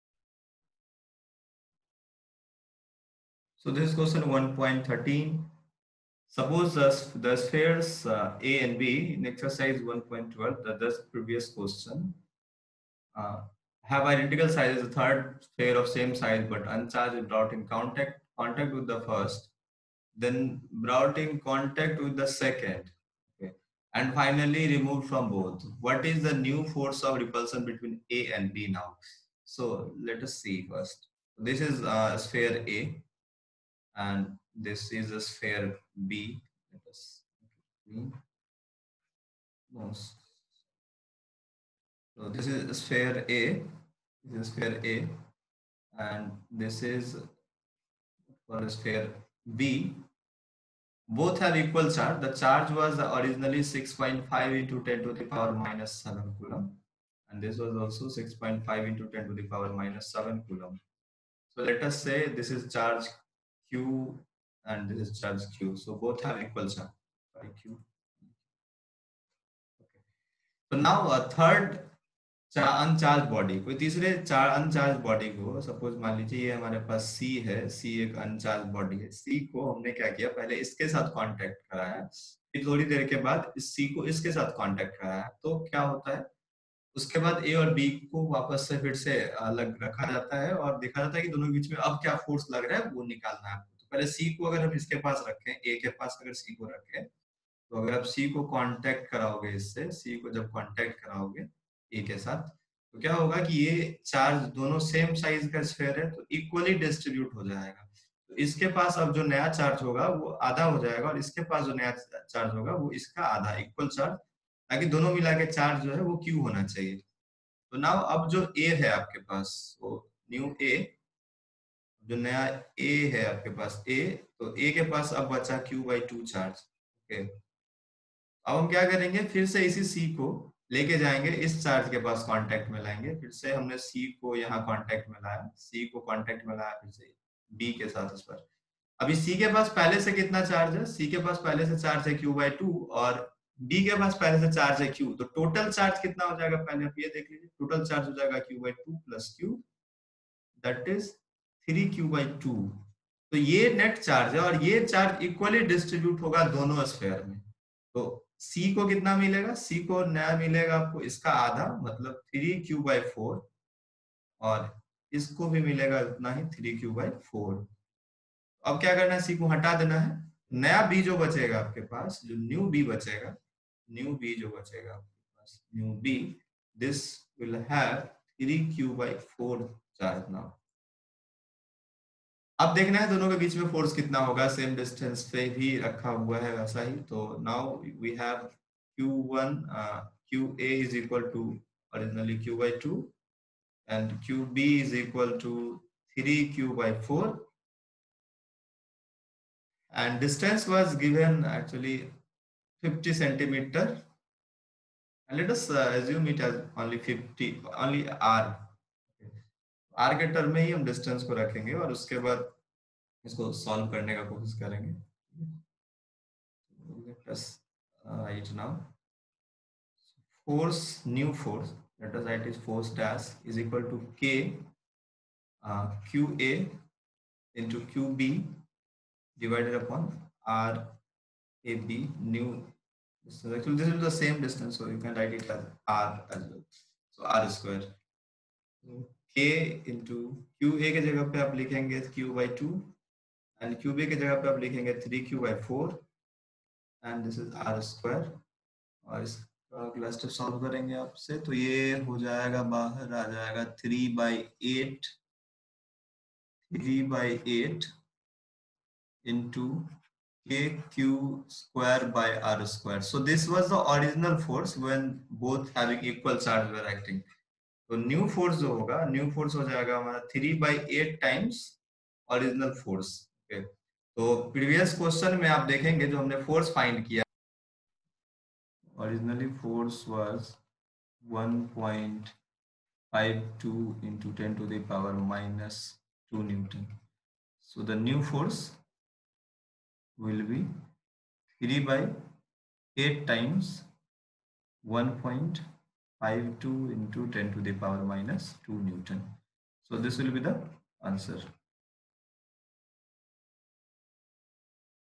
Q&A | Electric charges and fileds | NCERT | Q1.13
Solution of -
1.13 Suppose the spheres A and B in Exercise 1.12 have identical sizes. A third sphere of the same size but uncharged is brought in contact with the first, then brought in contact with the second, and finally removed from both. What is the new force of repulsion between A and B?